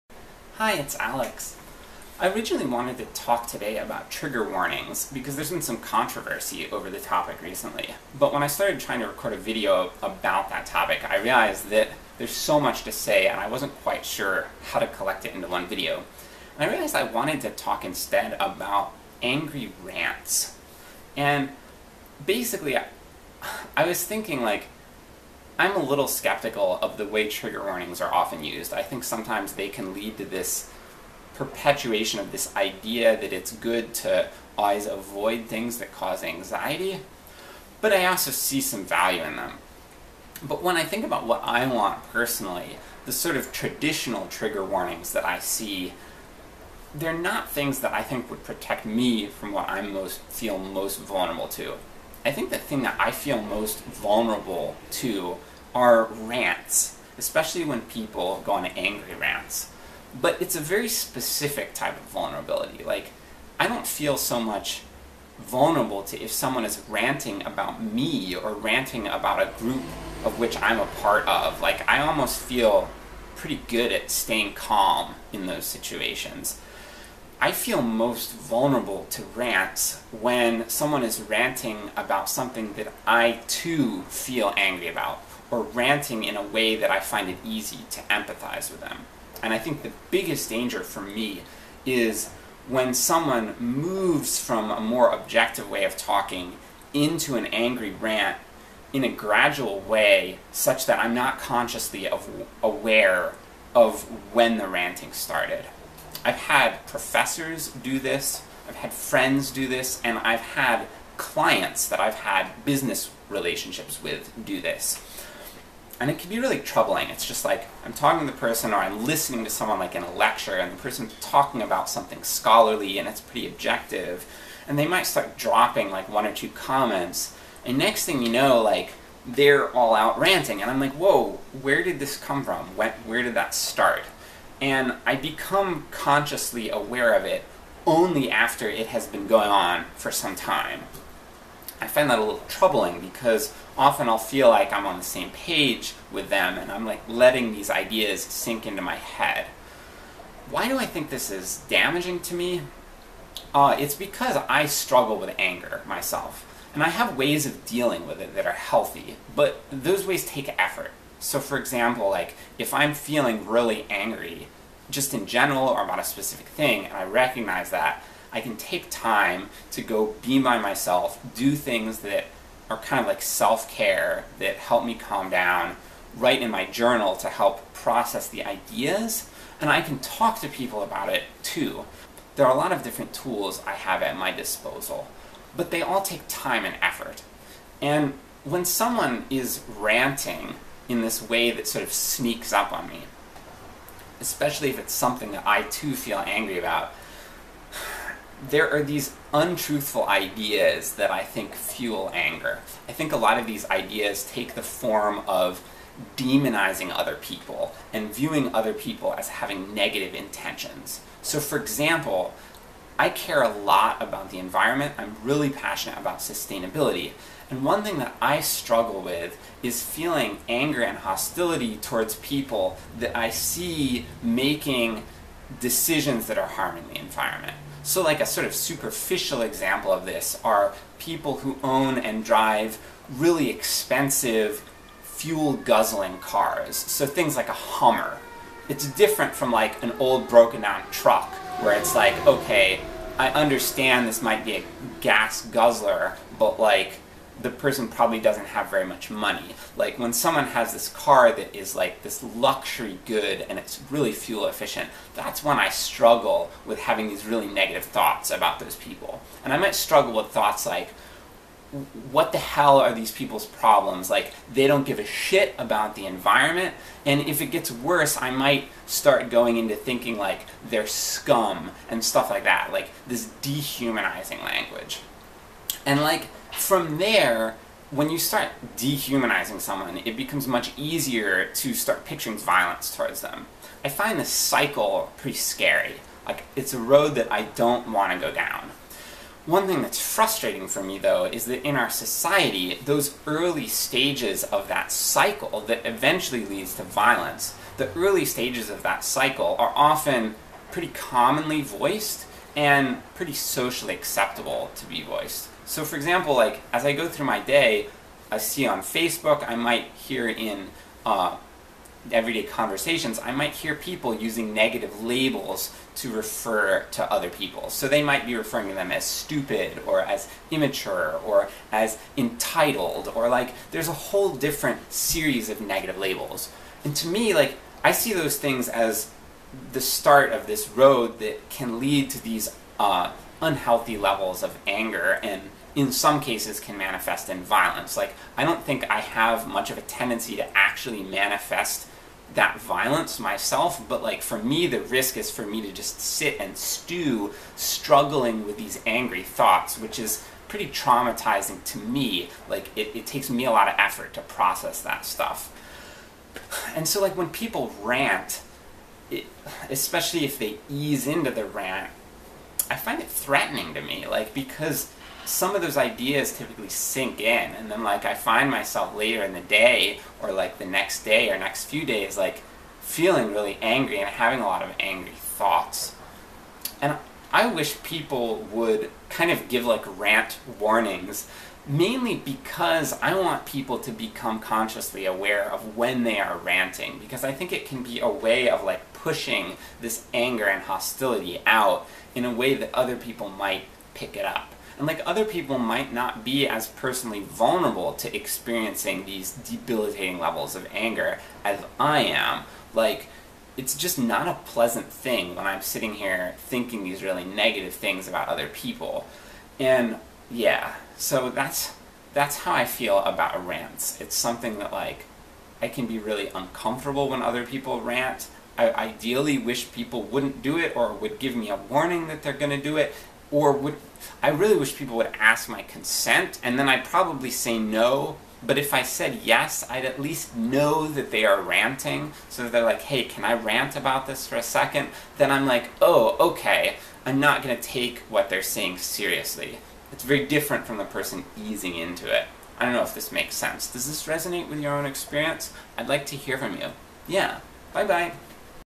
Trigger Warnings? How About Rant Warnings?
One of the things I find most troubling for my mental state is when people seamlessly transition from objective talk into an angry rant. I talk about how rants often contain untruthful ideas that can lead to unnecessary and unproductive levels of anger. I explain how I wish people would become more consciously aware of when they're ranting, and would ideally ask for consent before ranting to other people.